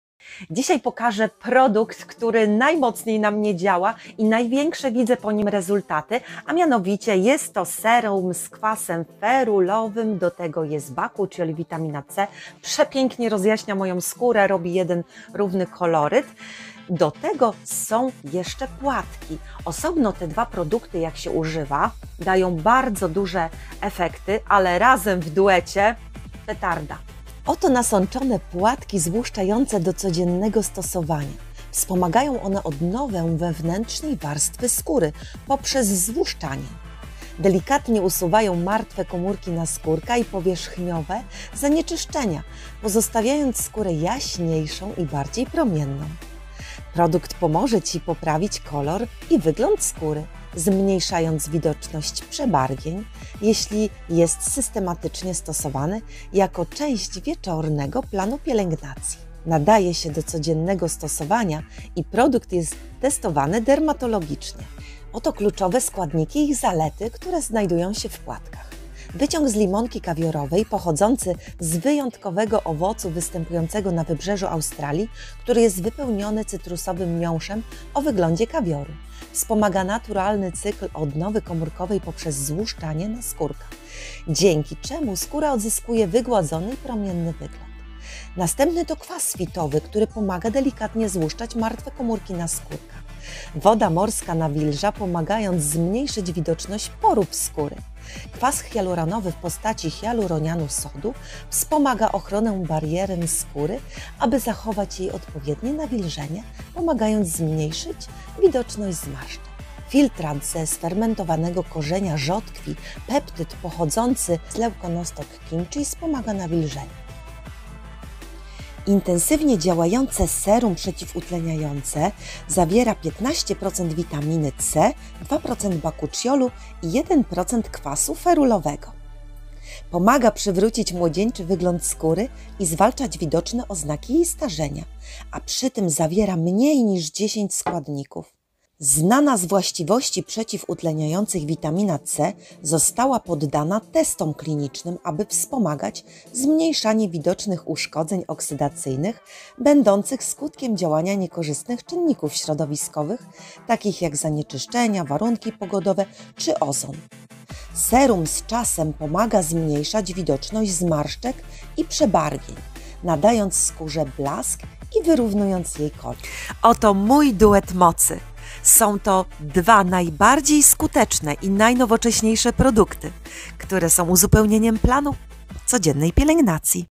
złoty środek na przebarwienia skóry i równomierny kolor skóry.
"Duet mocy" bo tak się nazywa innowacyjny preparat na przebarwienia skóry.
Na chwilę obecną czyli 1 wrzesień 2021r jest to najskuteczniejszy i najnowocześniejszy preparat.Efekty poparte badaniami klinicznymi.
Więcej na stronie produktu.